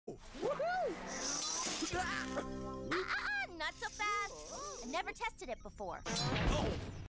(BAD PERSON) Gets Trapped By A Pick Up Tool, But He Falls Down Off A Trampoline
You can use this for someone you hate.

NOTE: I like Wild Kratts (to good users that hate Wild Kratts: I respect your opinion).